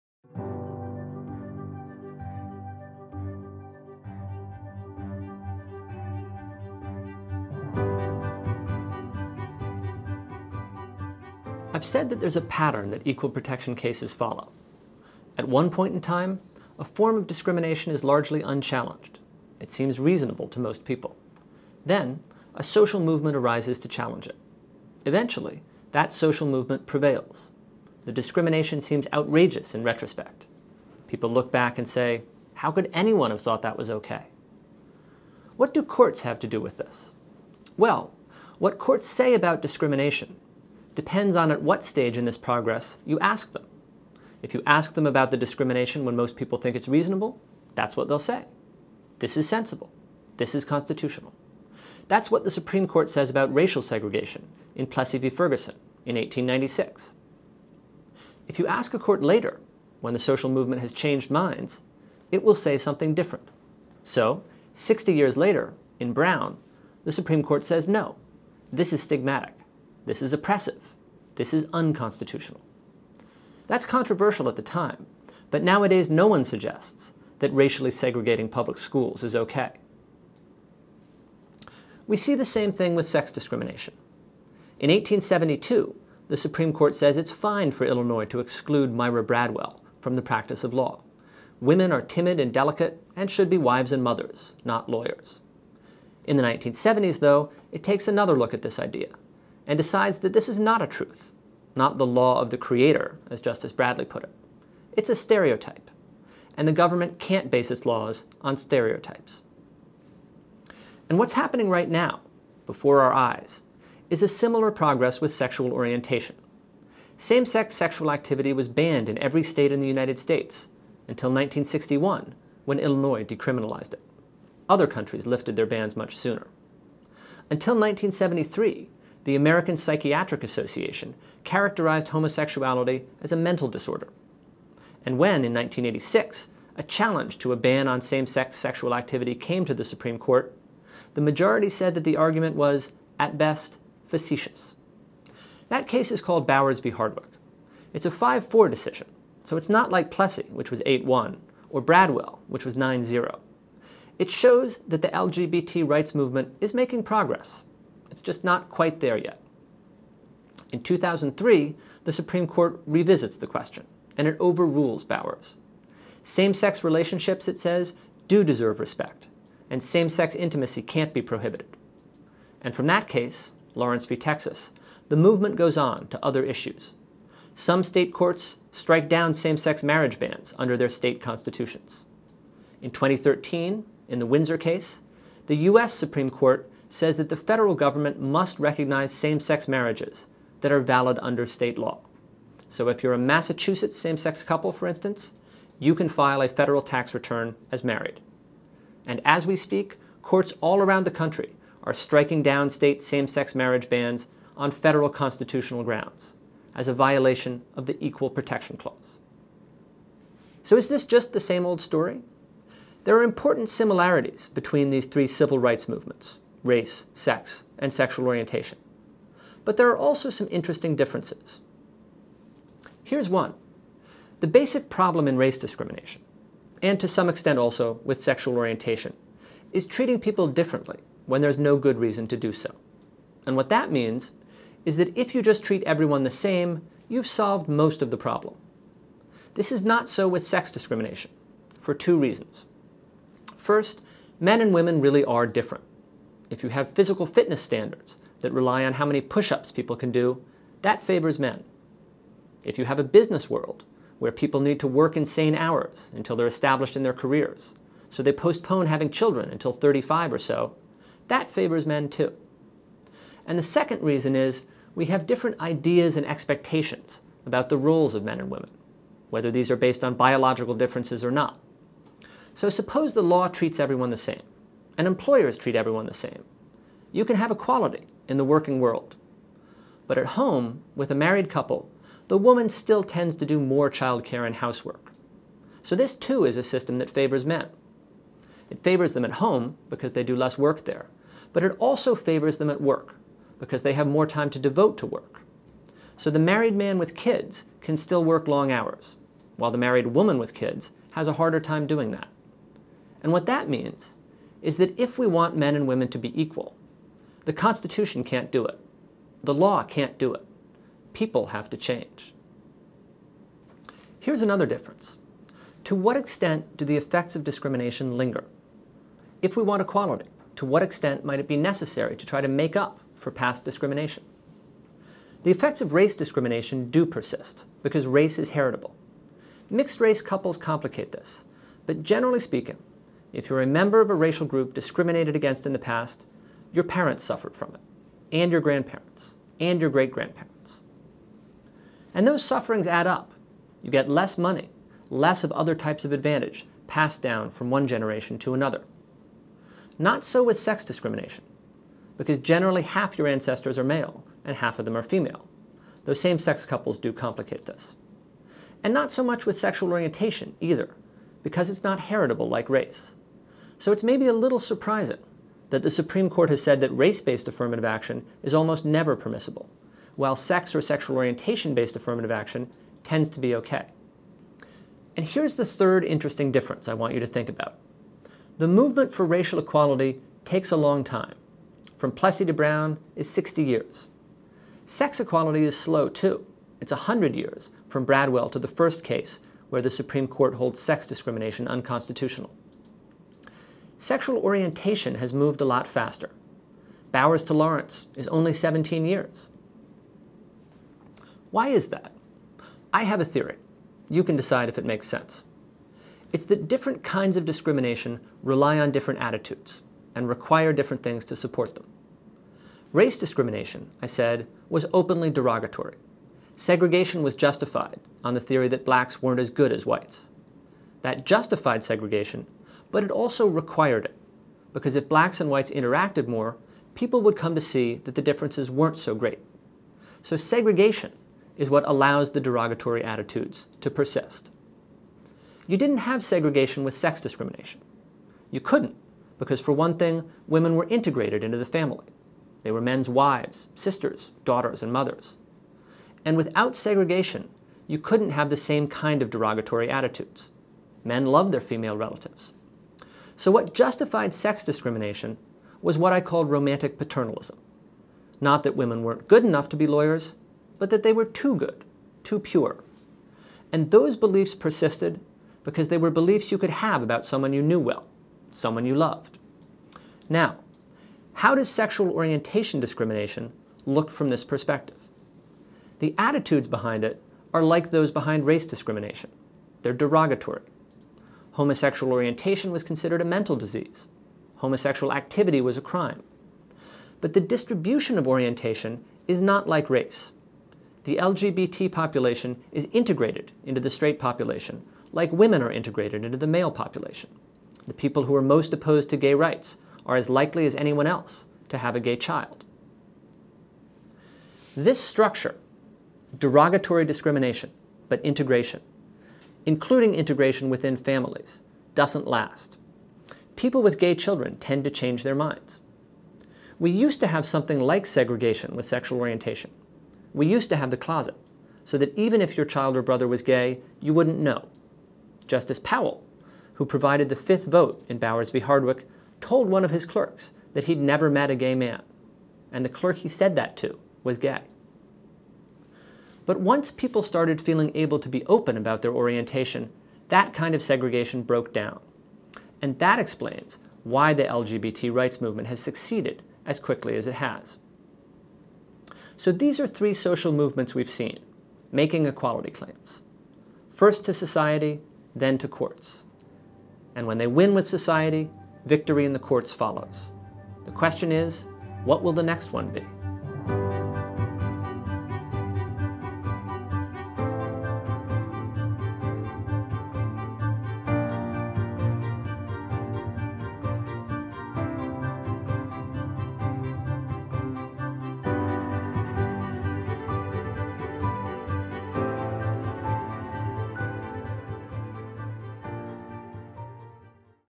7.3 Sexual Orientation
- A selected lecture from "Introduction to Key Constitutional Concepts and Supreme Court Cases" with University of Pennsylvania law professor Kermit Roosevelt III. For more civics education resources, go to AnnenbergClassroom.org